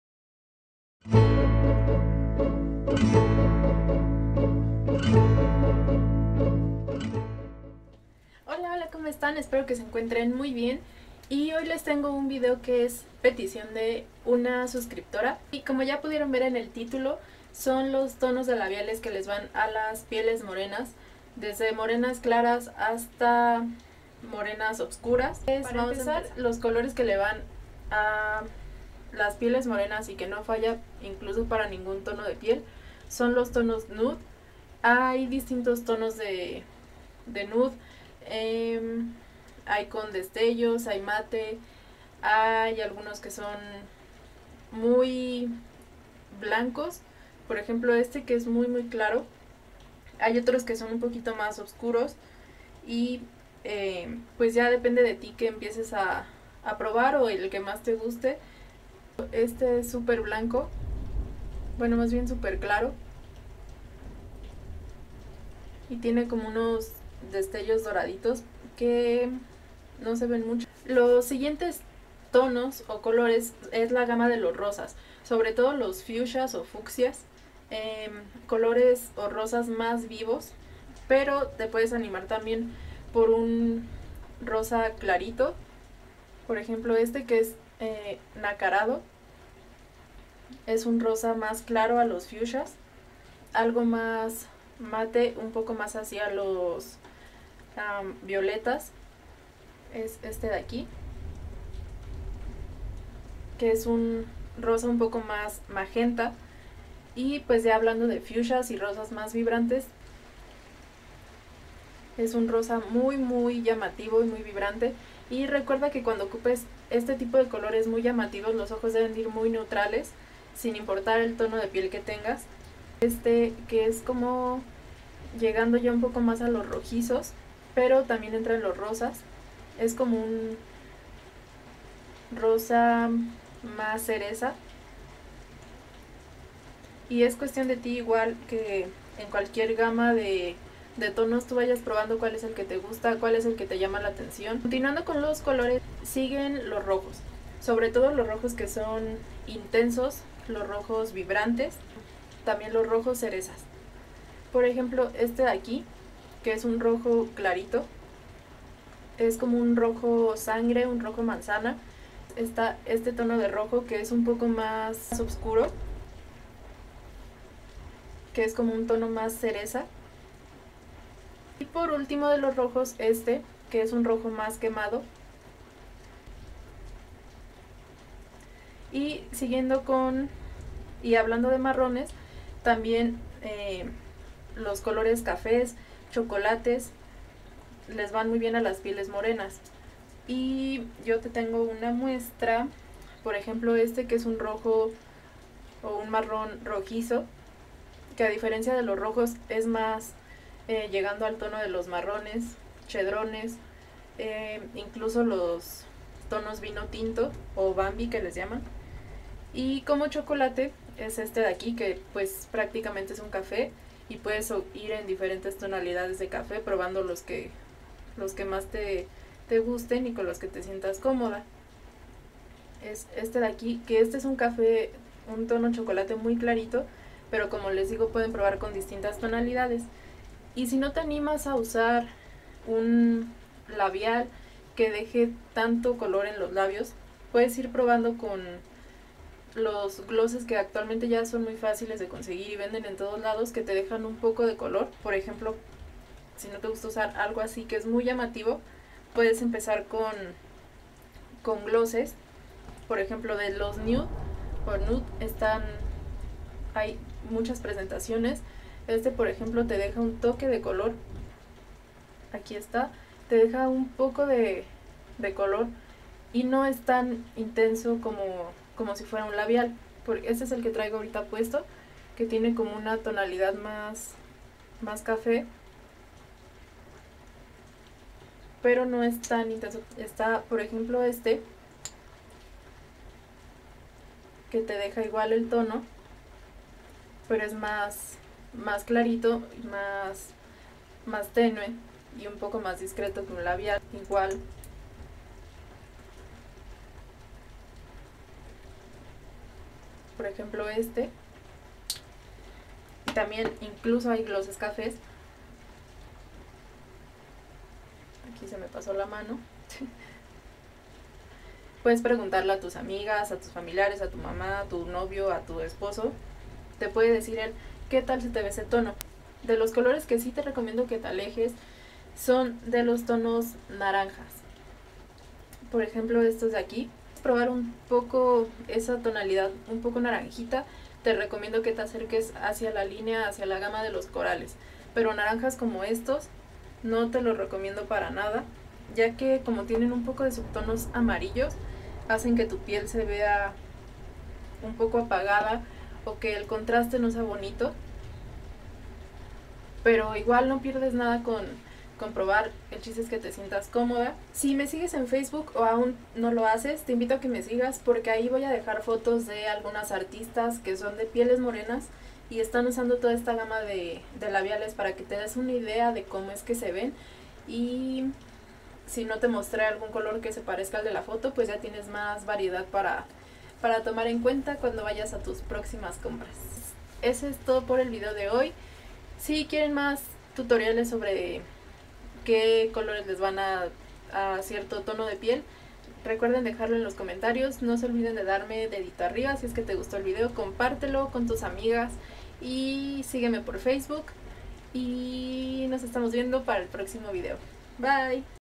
✱❀ Labiales para pieles morenas✱❀
Holaaa!!! Si eres de piel morena este video te sera de mucha utilidad!

Ayudame compartiendo este video y dandole manita arriba